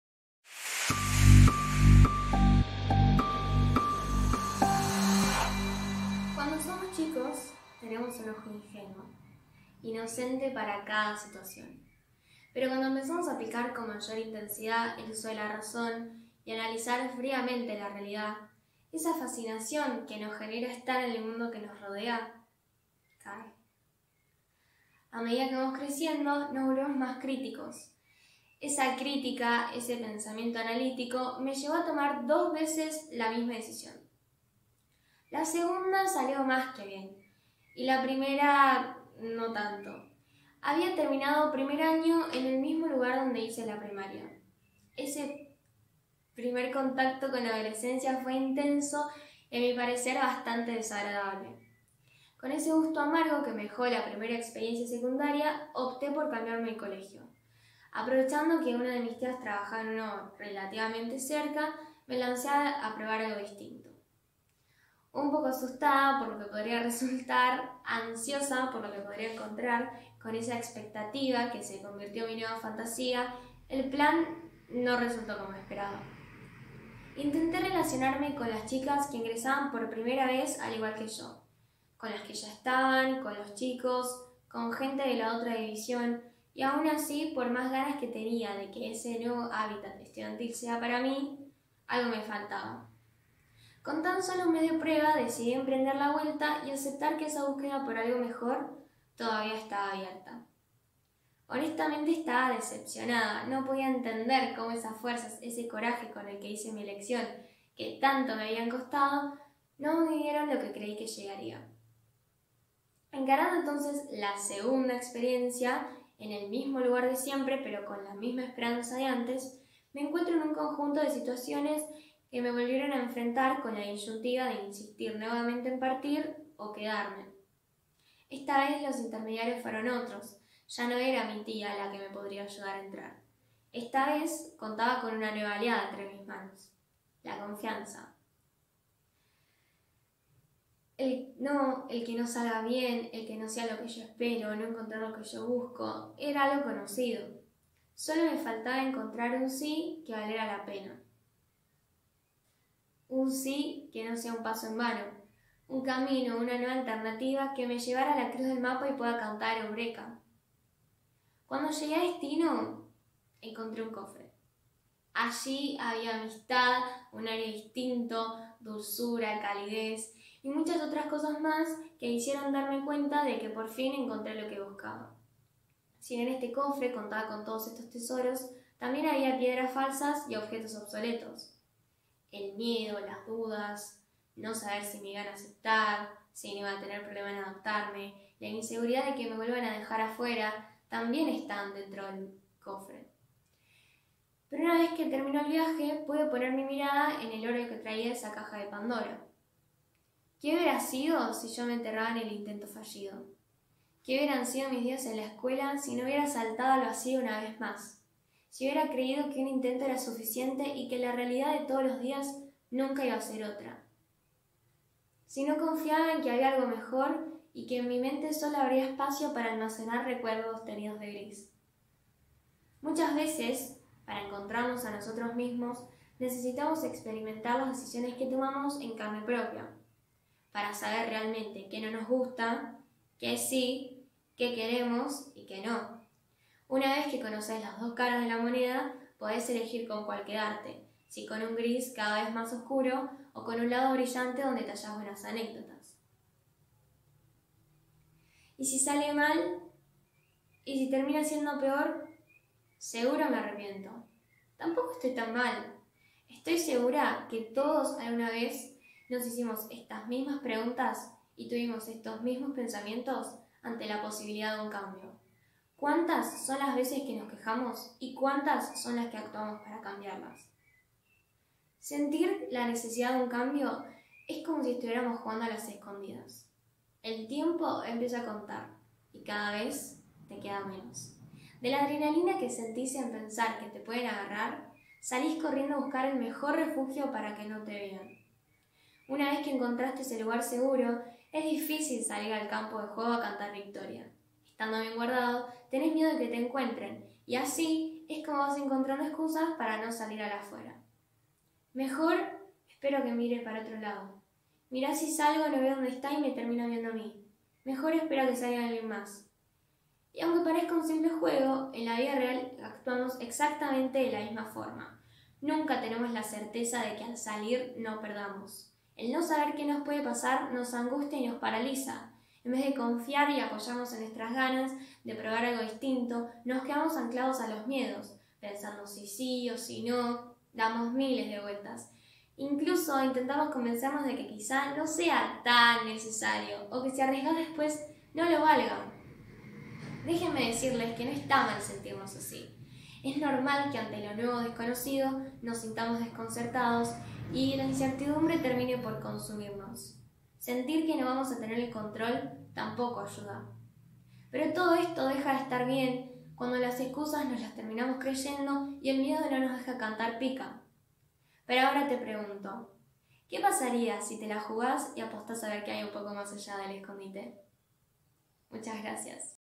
Mas allÁ del escondite - Johana Ianuzzi - Clubes TED-Ed
ClubesTedEd - COLEGIO SANTA TERESA DEL NIÑO JESUS - Licencia: 2020
Titulo: Mas allÁ del escondite
Dada por: Johana Ianuzzi